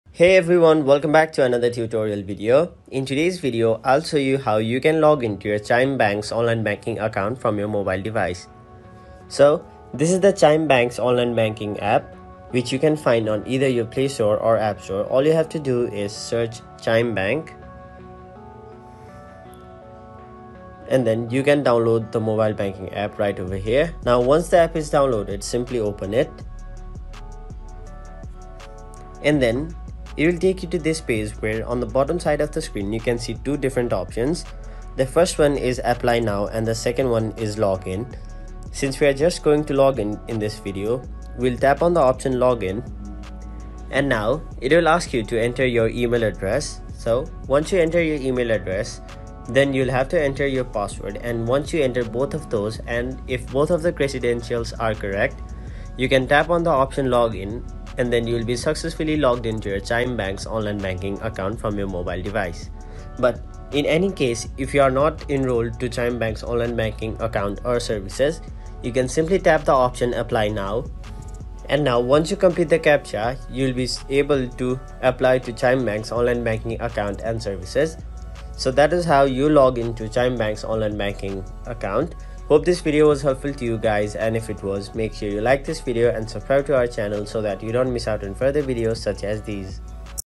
Chime Mobile Banking App Login | Sign In Chime
This video guides you in an easy step-by-step process to login to your account on Chime mobile banking app.

This YouTube Channel "Tech Quest" is not the Official Support of the content provided. These videos are only educational and informational.